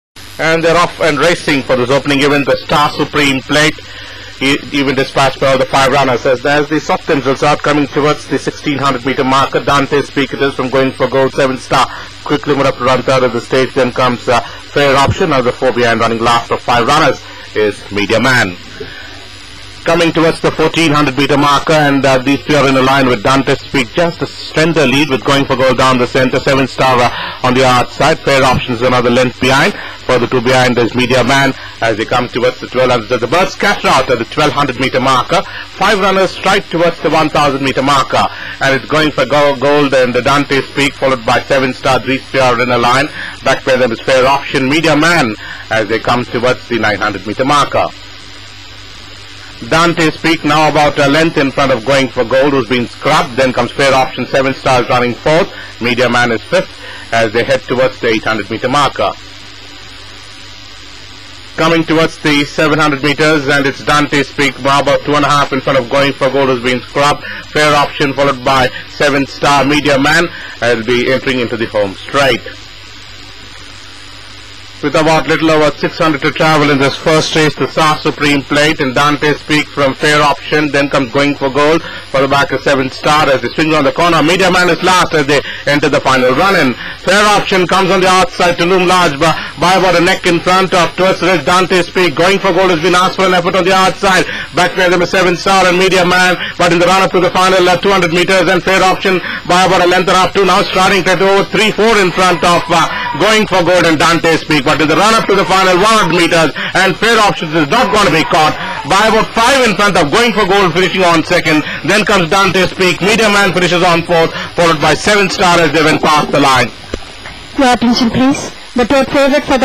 Fair Option wins going away at Bangalore Summer 2009 under a back breaking 62.5 KG
Fair Option (Inner City-Factual) wins easily over 9 Furlongs for trainer S Ganapathy and jockey B Prakash.